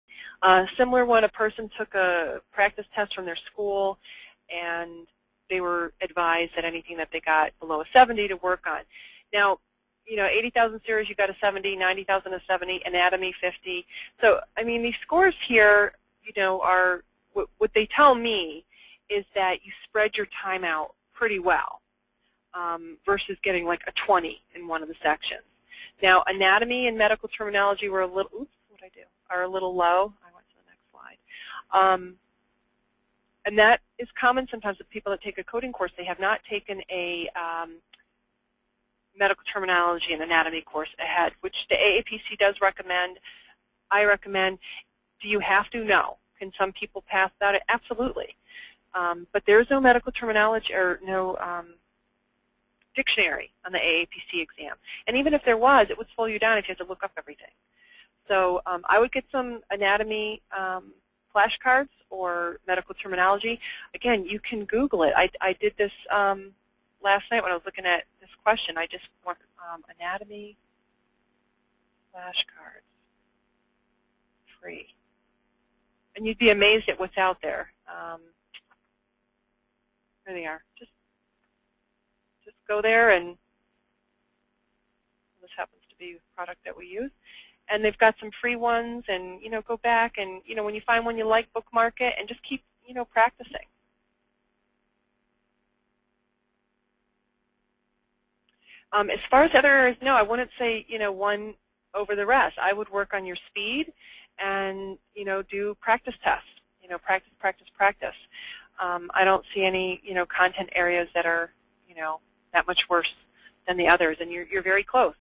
How to Pass the Medical Coding Certification Test Exam?
A medical student asked:
I just took a post test exam from my school and the areas that I got 70 and below is what I was told to study, they were

80000 70%,
90000 70%,
Anatomy 50%,
Anesthesia 63%,
Coding Guidelines 67%,
ICD-9-CM 60%,
Medical Terminology 63%,
Practice Management 50%.

Any advice on what I should concentrate on first, or should I just go down the line and review each section?

Sure! Watch the video.